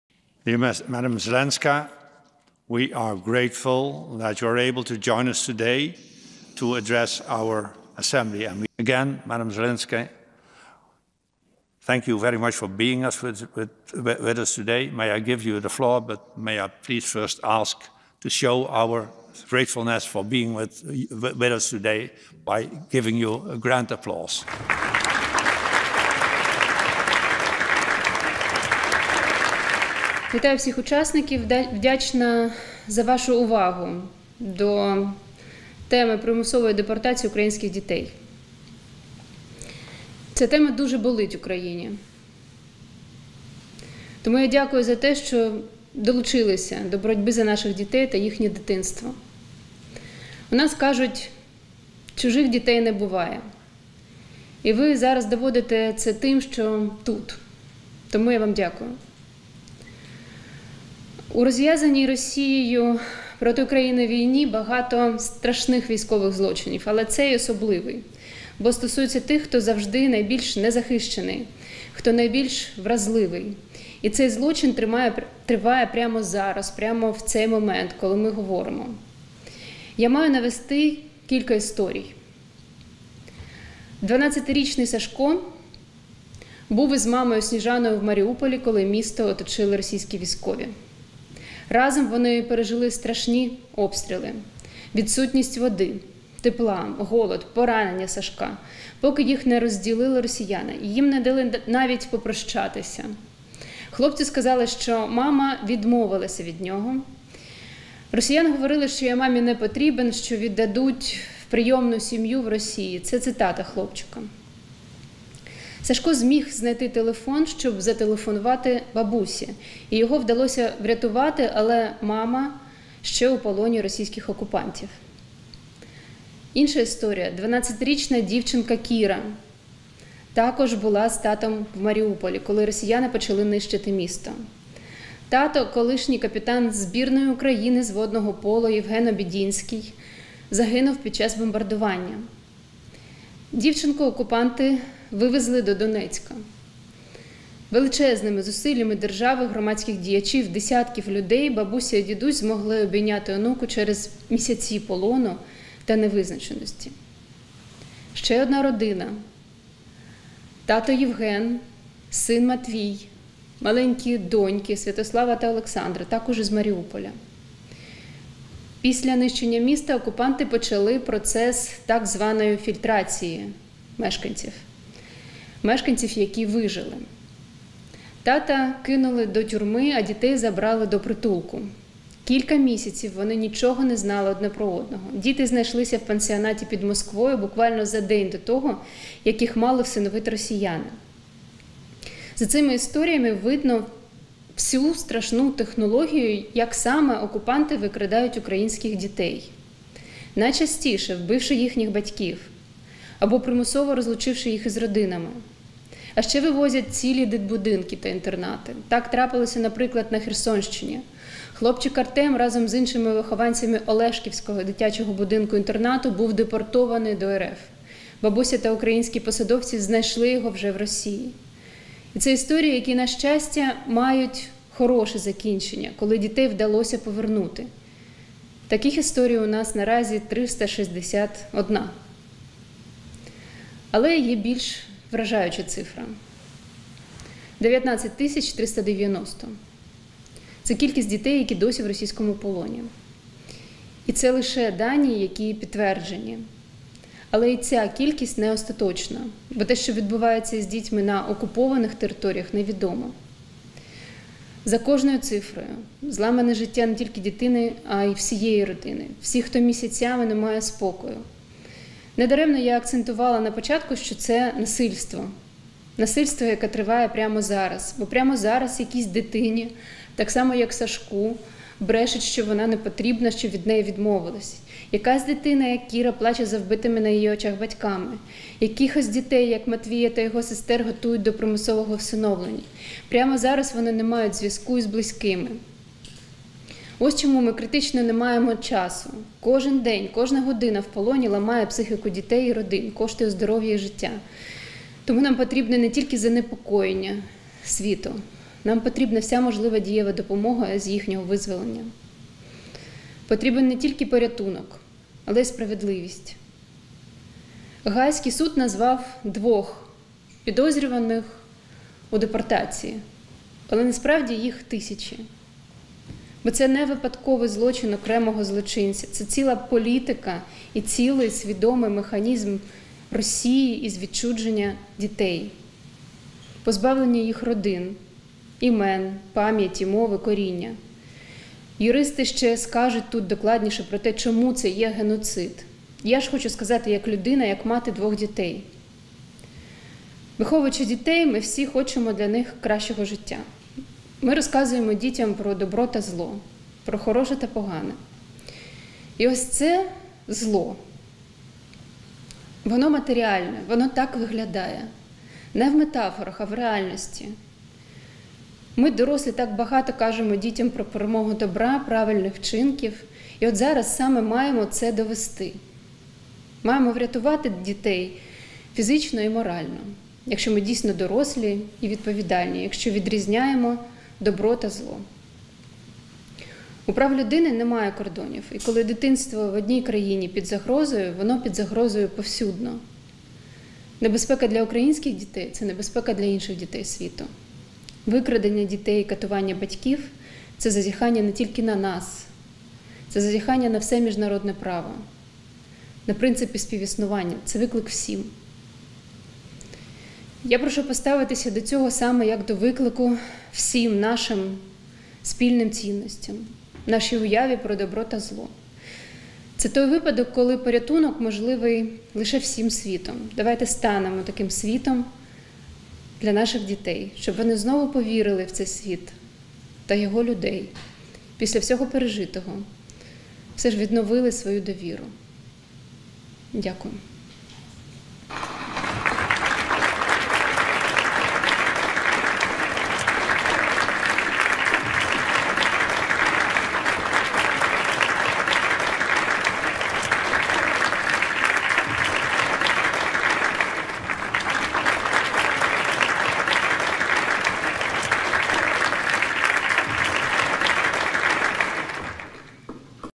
Олена Зеленська виступила на дебатах ПАРЄ щодо примусово переміщених Росією українських дітей
Перша леді України Олена Зеленська взяла участь у дебатах, які відбулися за терміновою процедурою на пленарному засіданні другої частини сесії Парламентської асамблеї Ради Європи.
Під час засідання було заслухано доповідь та ухвалено проект резолюції на тему: «Ситуація з українськими цивільними особами, зокрема з дітьми, примусово переміщеними або переселеними до РФ або на українські території, що перебувають під фактичним контролем РФ».